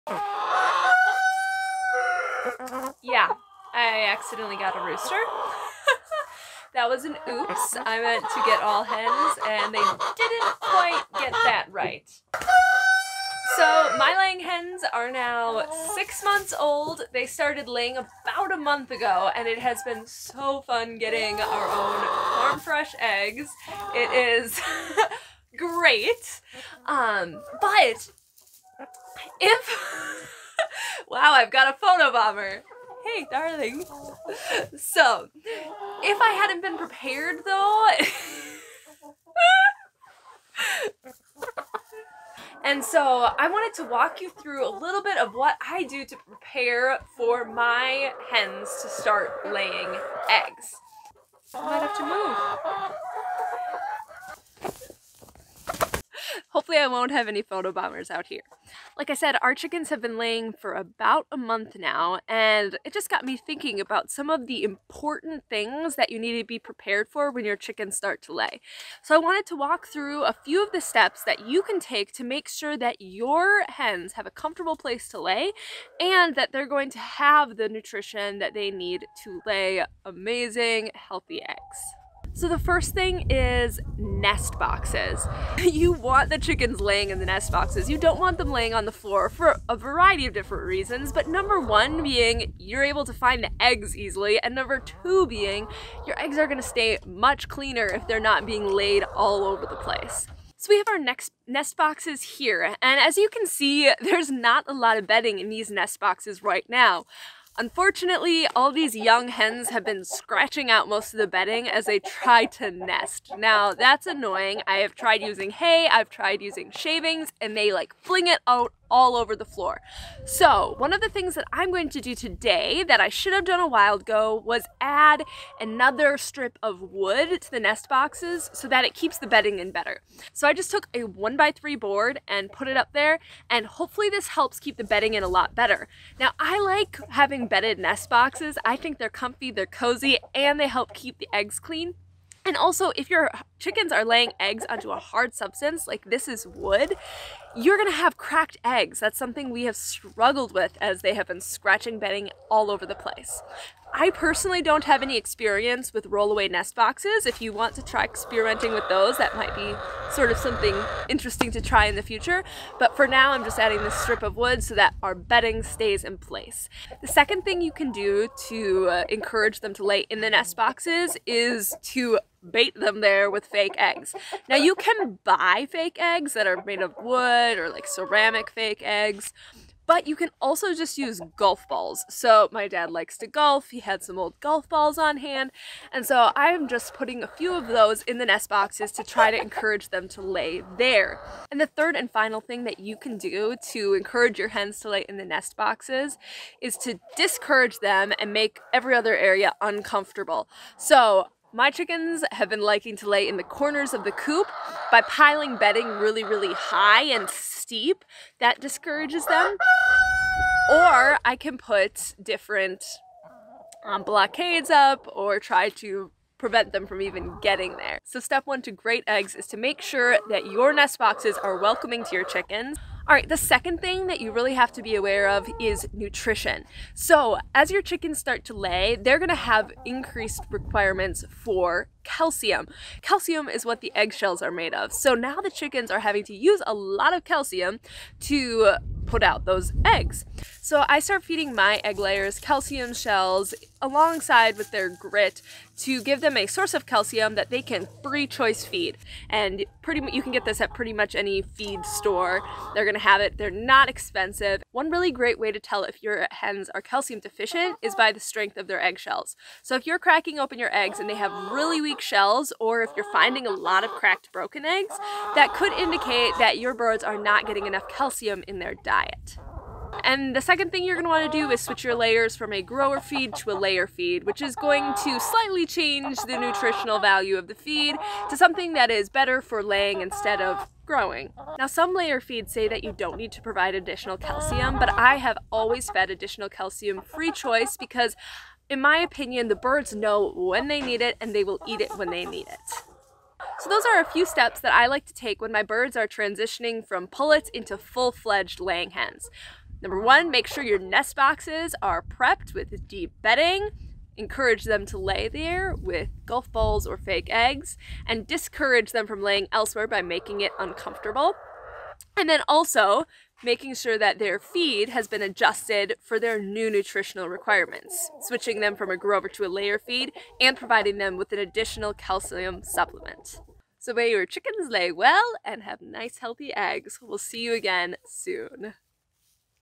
What To Do Before Your Hens Start Laying
You’re counting down the weeks and it could be any day now. As you thoroughly search your coop, you wonder…when will you FINALLY get your first egg?
In this video, I talk about a few important things to consider before your hens start to lay.